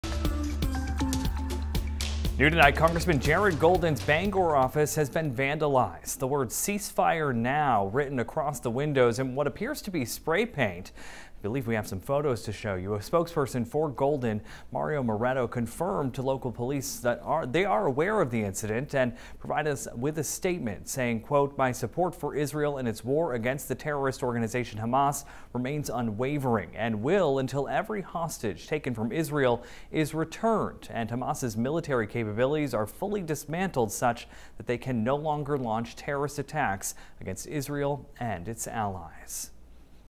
Rep. Jared Golden's Bangor office vandalized
The words "CEASEFIRE NOW" were written across the windows of the office in what appeared to be spray paint.